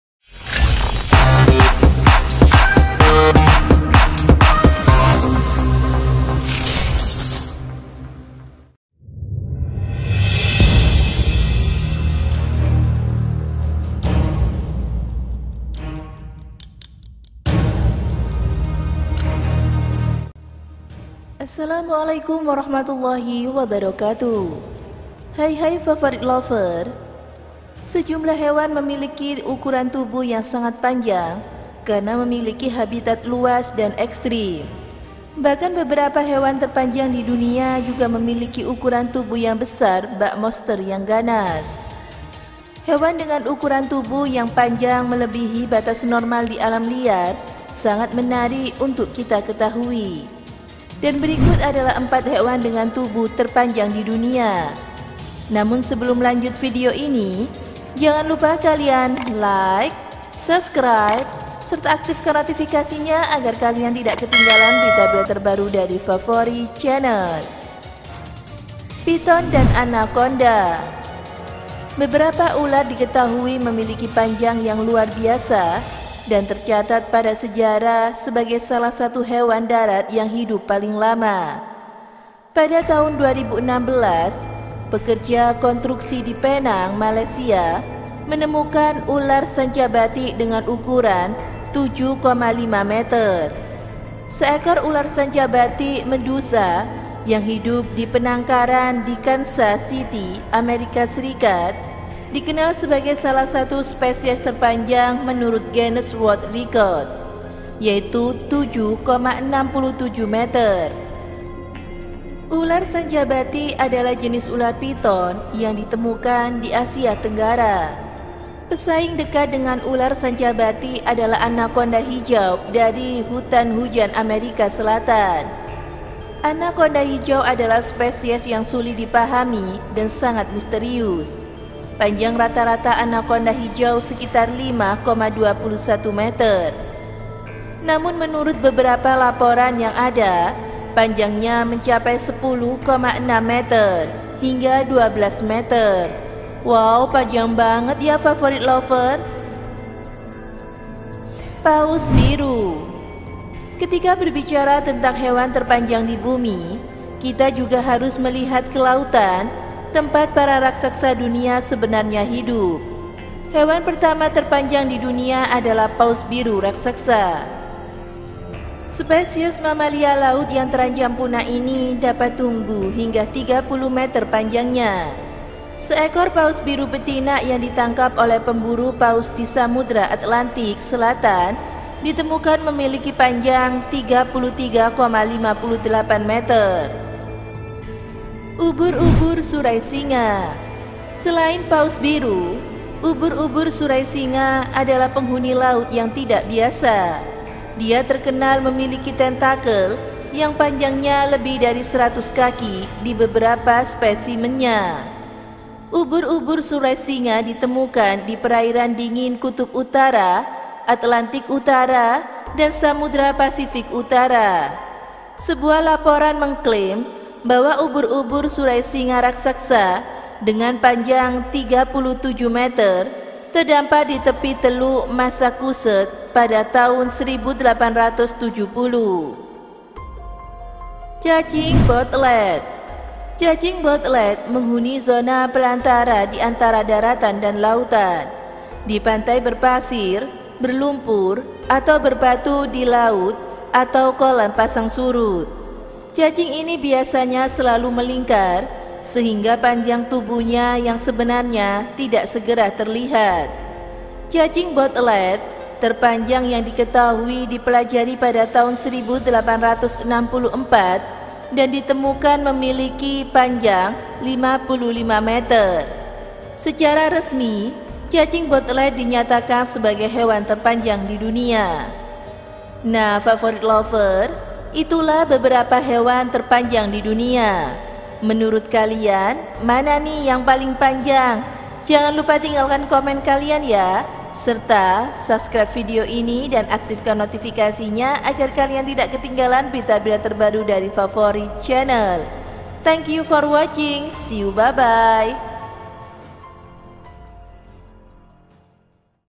MOSTER YANG SESUNGGUHNYA! Inilah 4 Hewan Penguasa dan Terpanjang Di Dunia, No 3 Sering Kita Temui.
Berbagai jenis makhluk hidup tepanjang di dunia diketahui hidup di lautan. yang diperkirakan masih ditemukan di zaman kita ini. Dan berikut adalah hewan terpanjang di dunia:

1. ubur-ubur surai singa
2. paus biru
3. cacing bootlace
4. ular piton
5. Gurita raksasa
6. hiu penjemur